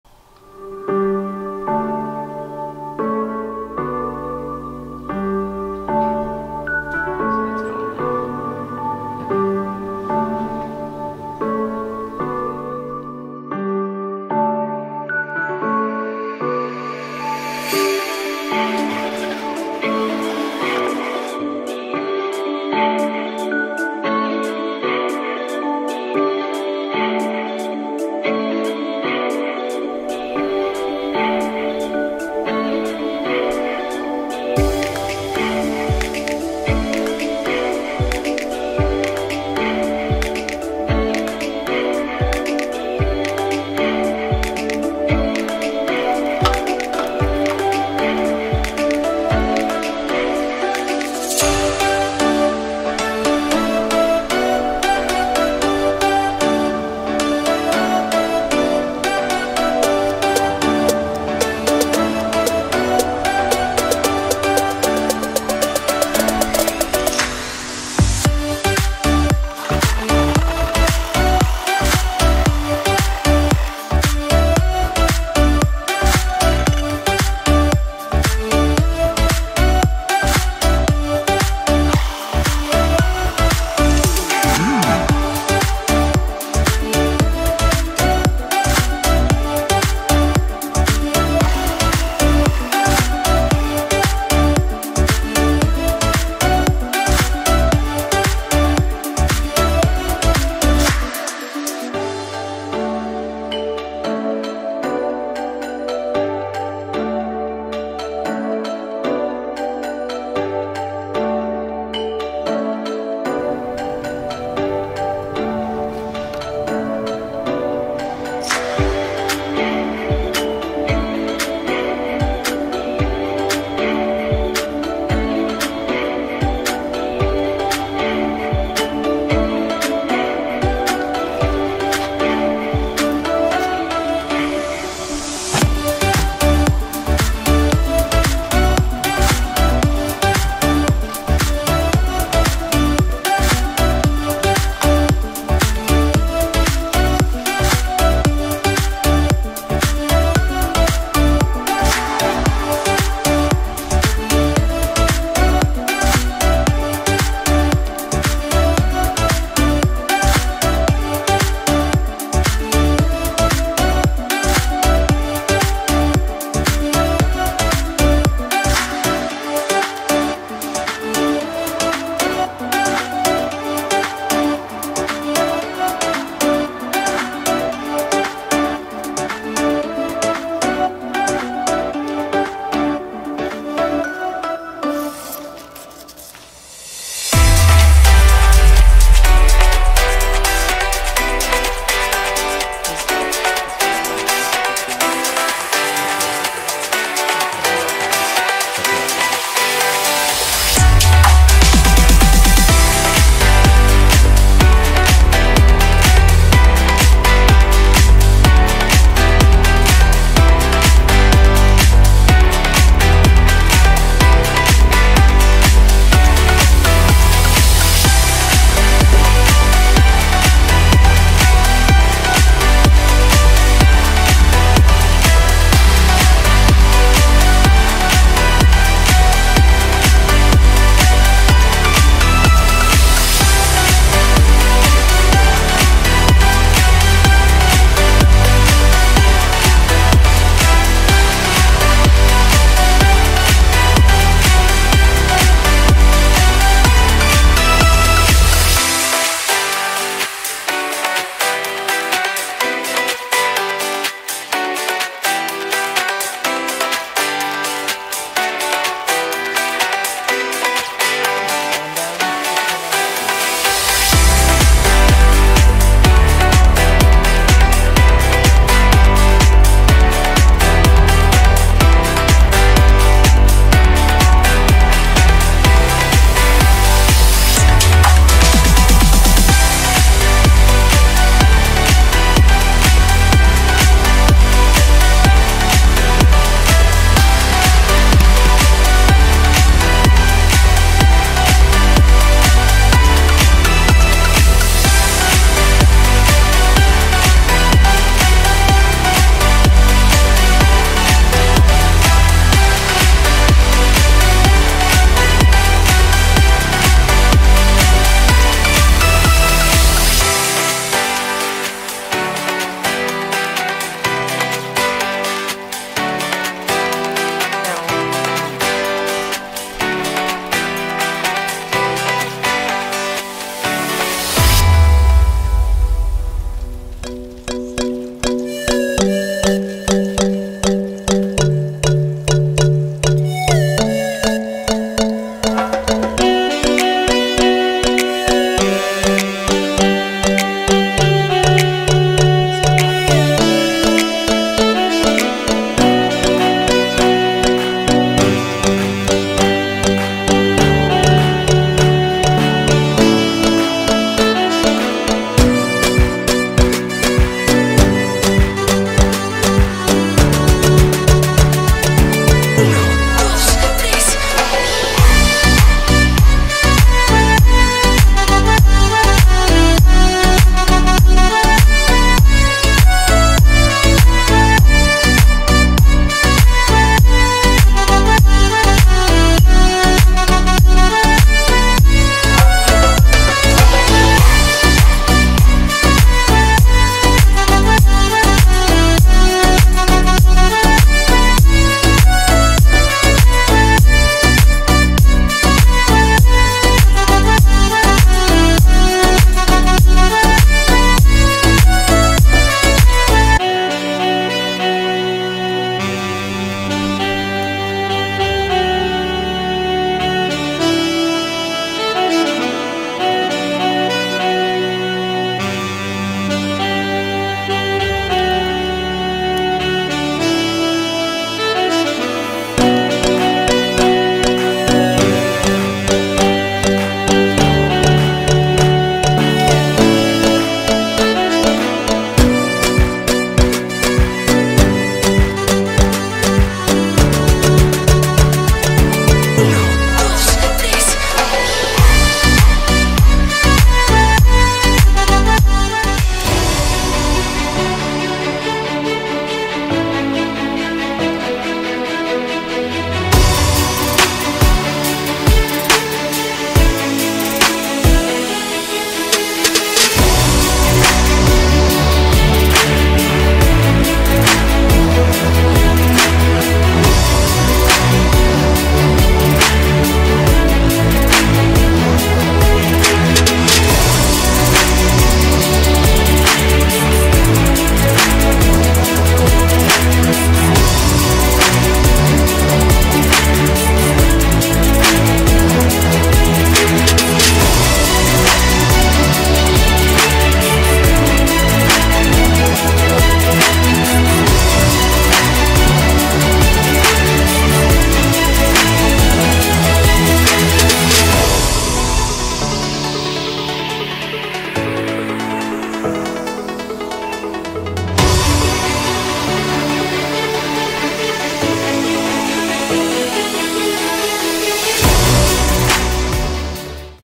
From cat lamp... to pothos plant trellis!
I repurposed an old lamp into a beautiful trellis to hold my golden pothos plant. I LOVE the way it turned out!